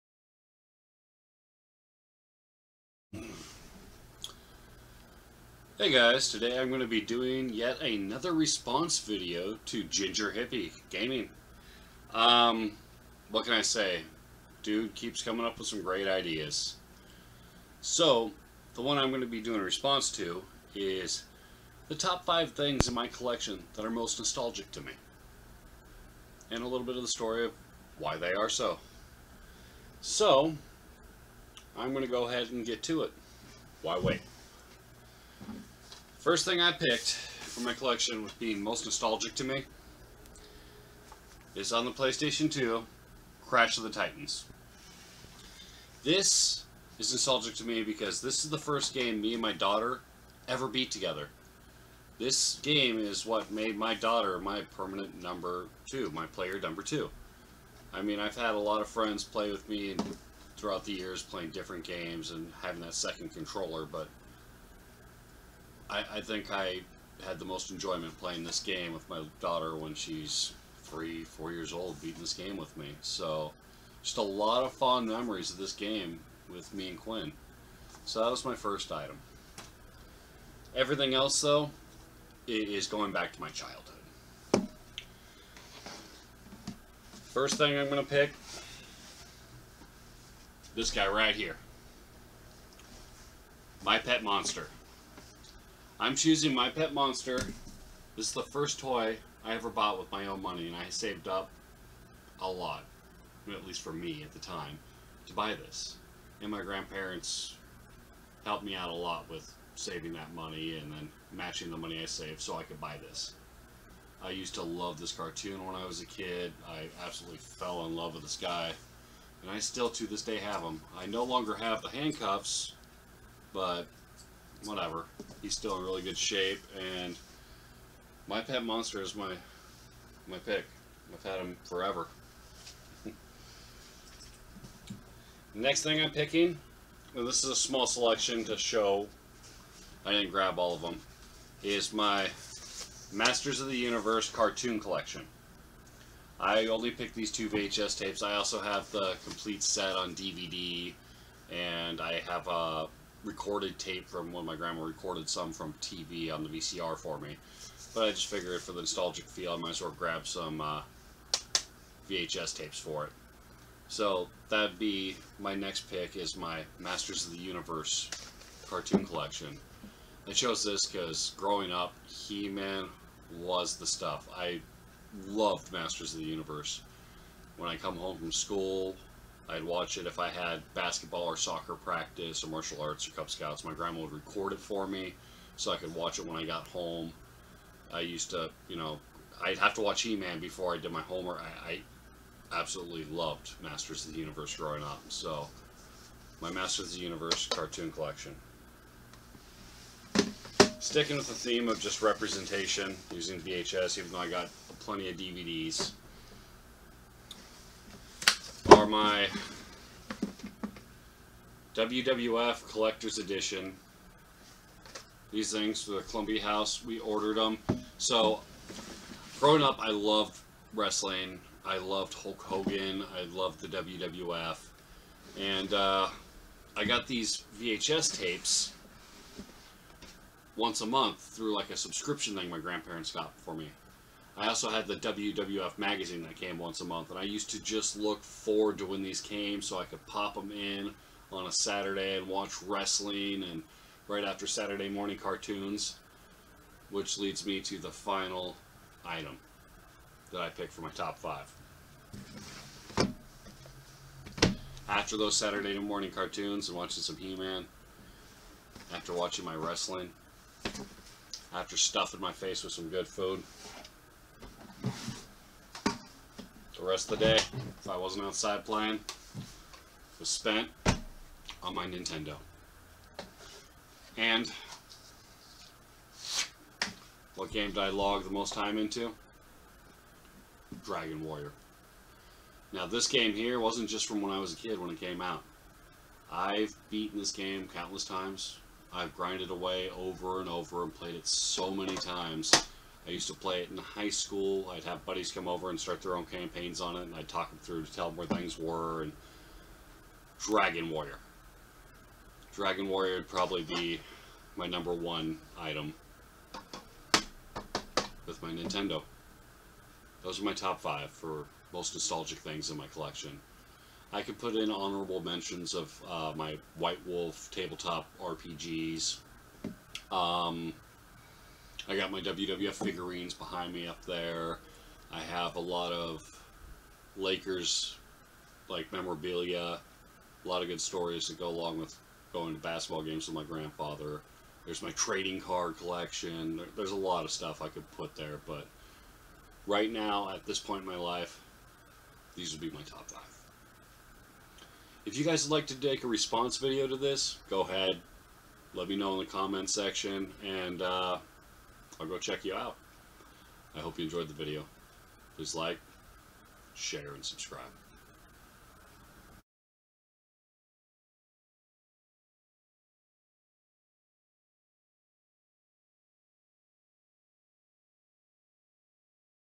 My Top5 Nostalgic Items in my Collection (Ginger Hippy Gaming Response)
Overgrown Manchild's Collection #61
Yet another Ginger Hippy idea I just had to steal, lol. What can I say, I had to join the fun on this one. Plus I got to take a stroll down memory lane.